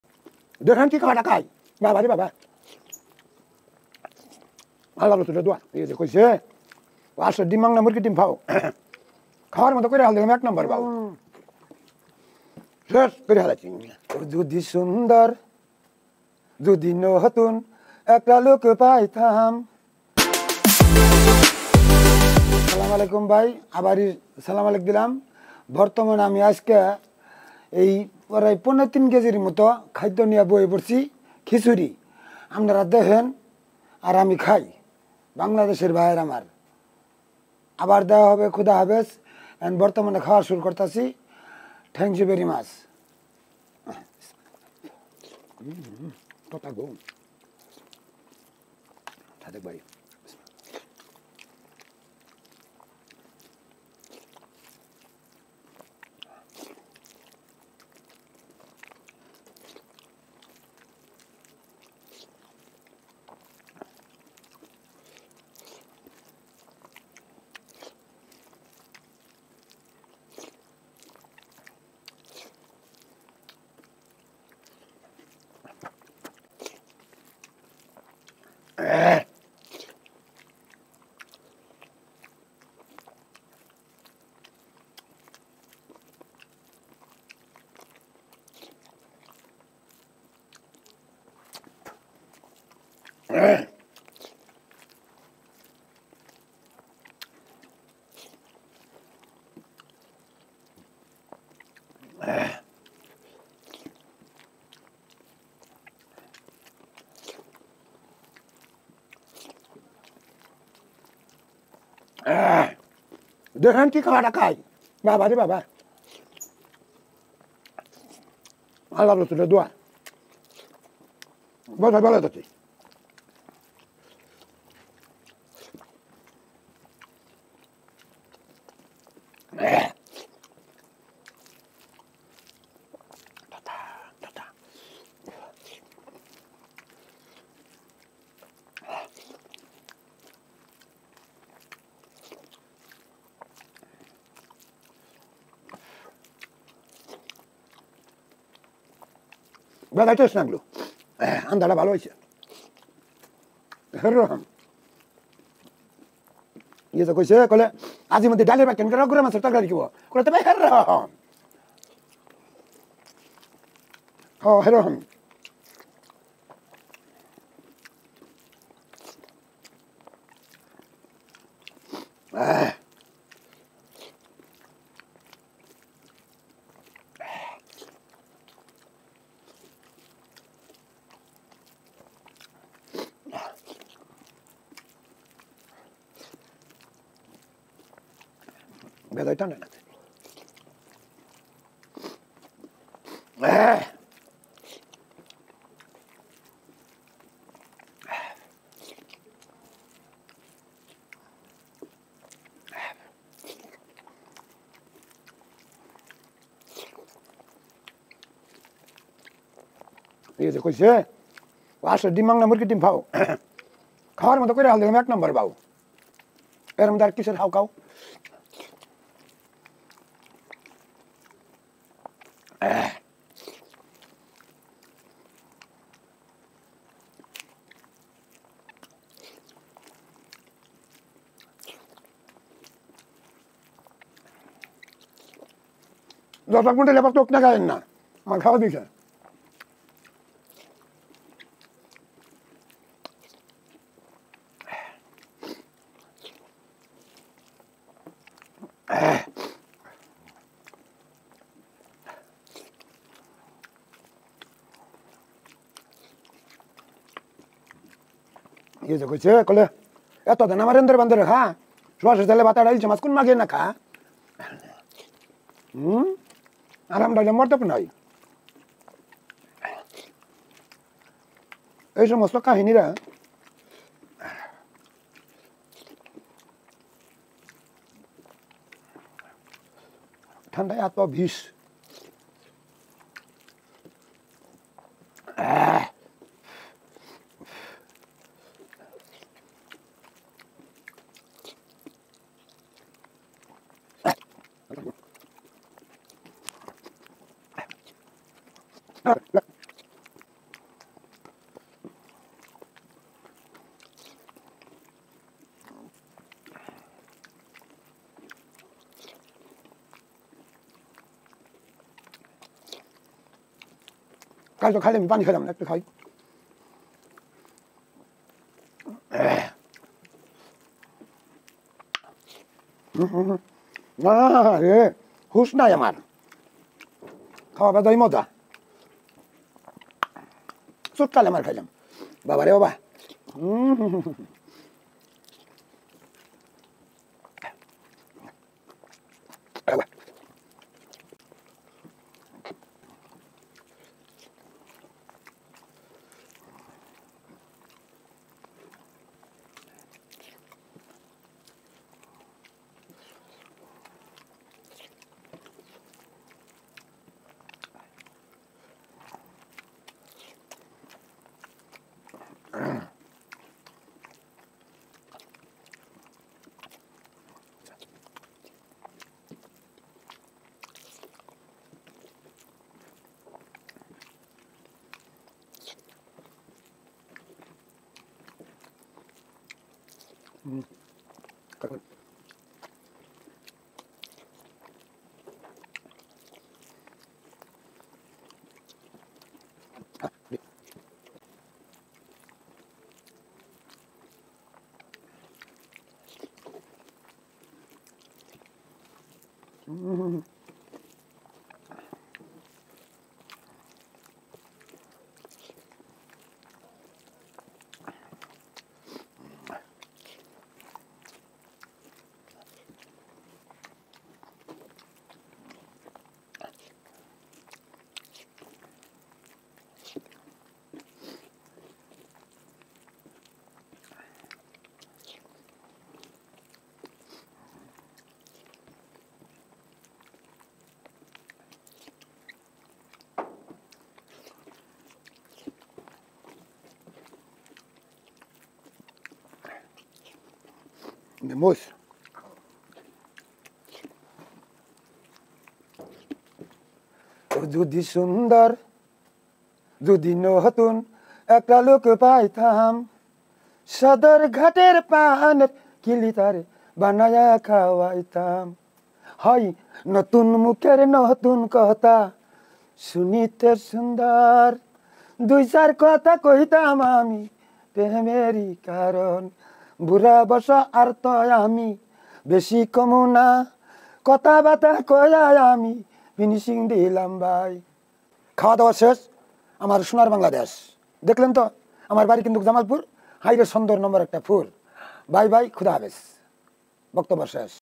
Eating My Favorite Khichdi🤤|| 70+ Years Old Man Eating Kichuri On Hungry Monster|| ASMR
⚠️ Warning ⚠️

Please Help Us & Support Us.

Thanks all supporters & viewers.